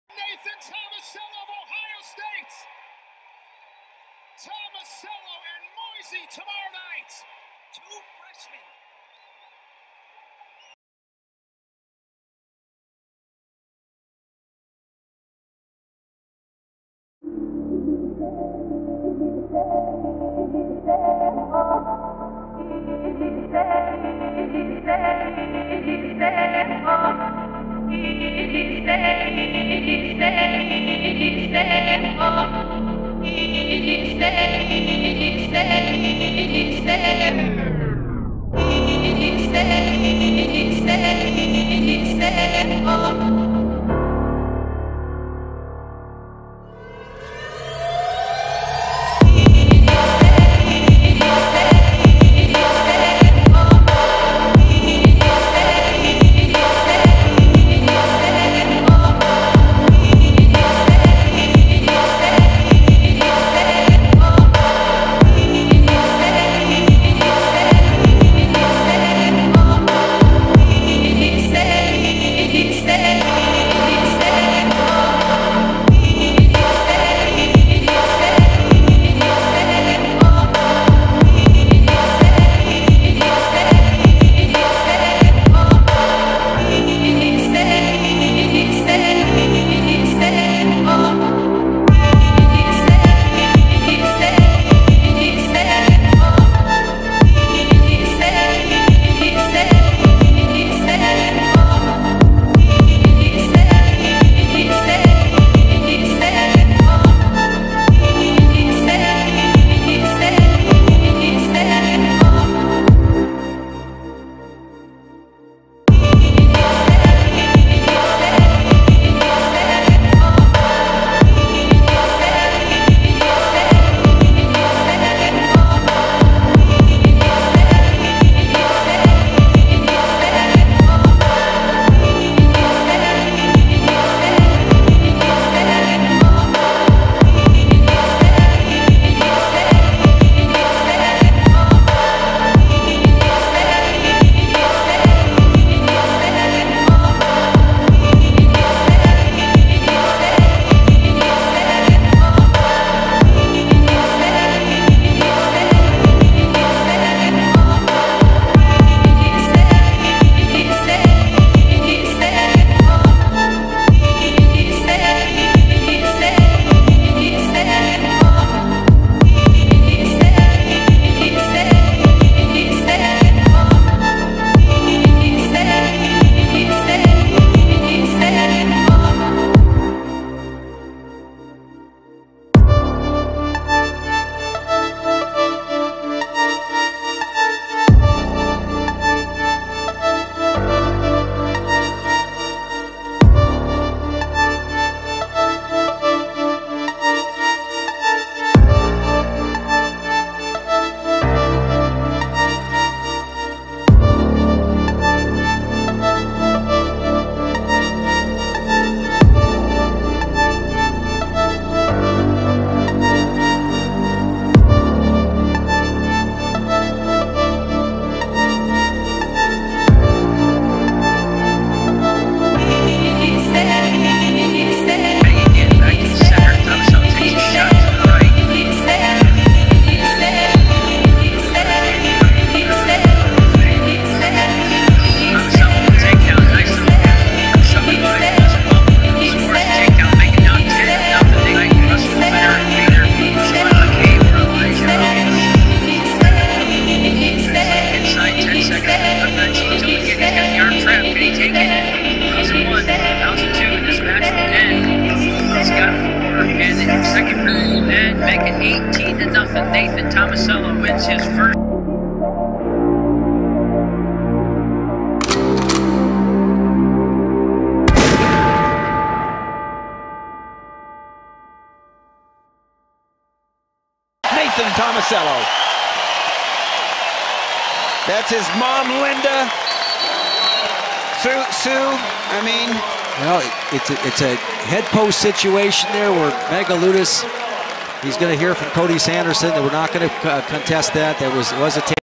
Nathan Tomesello Highlights
Ohio State, 2x All Amriacan, Current Junior, 2015 Ncaa Champ, @015 and 16 Big Ten Champ, Nathan Tomesello